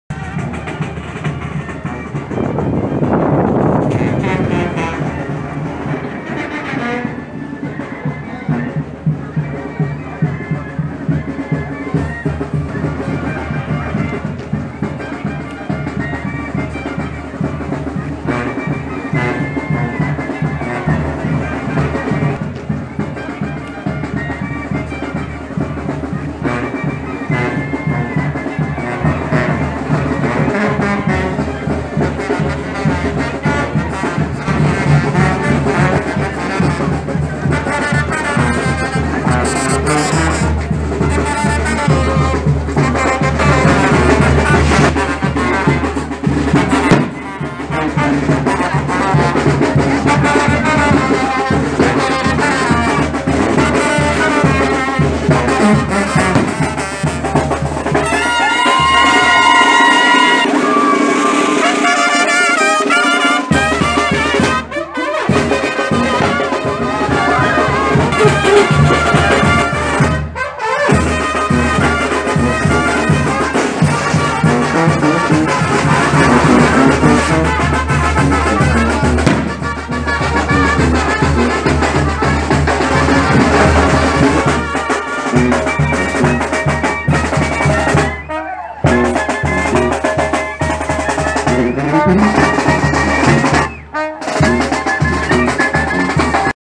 Chinelos Día de San isidro 2019
Esta danza es fiesta a San Isidro,
Parroquia de San Isidro  De Fiesta
Miércoles Jueves Viernes Sábado y Domingo